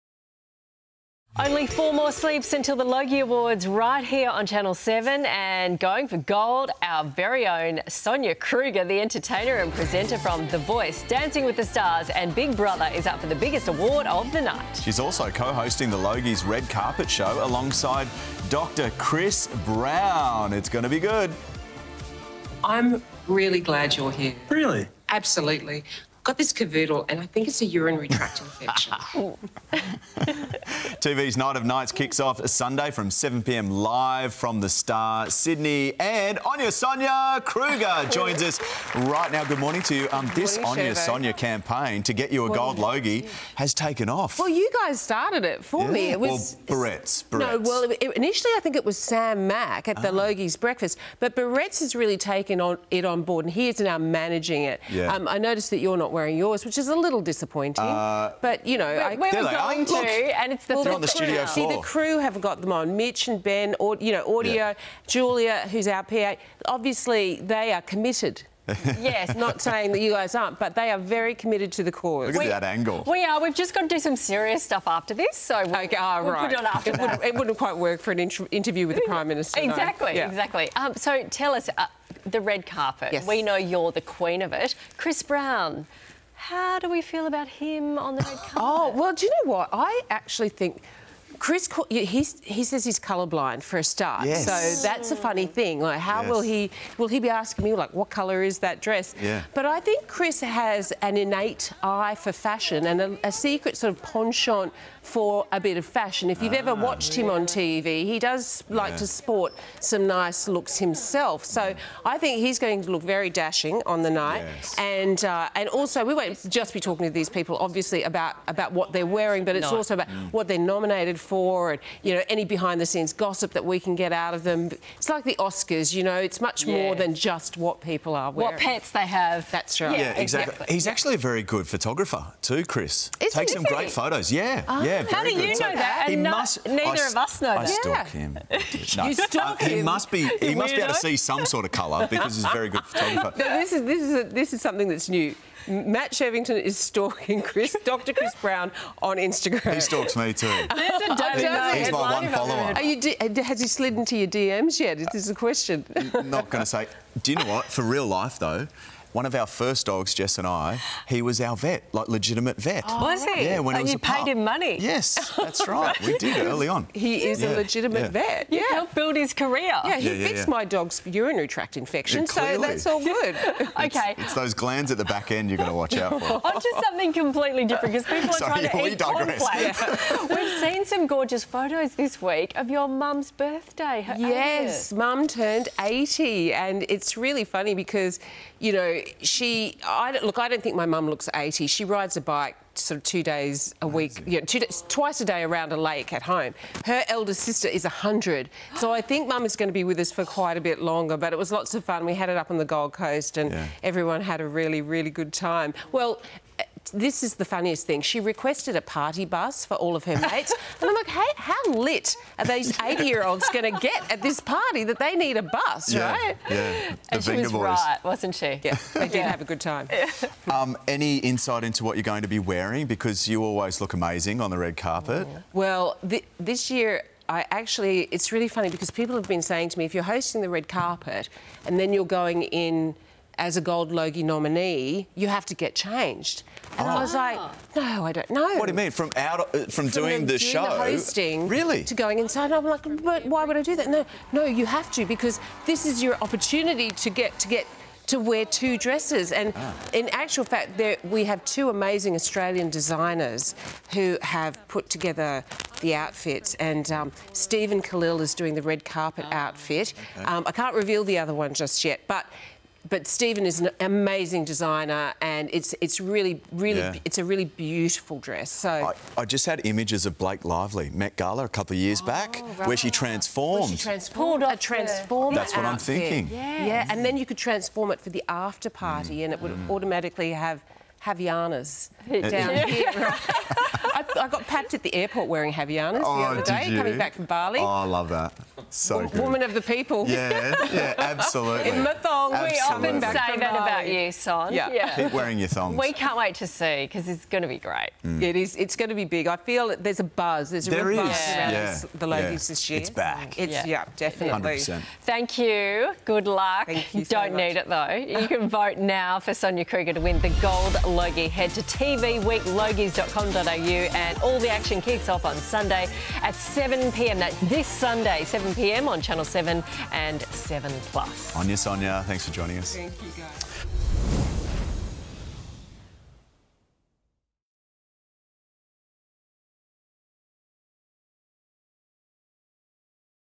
Onya Sonia: the campaign to get Sonia Kruger the Gold Logie
With the 65th TV Week Logie Awards coming up, there's a campaign to get The Voice Australia host the night's top gong.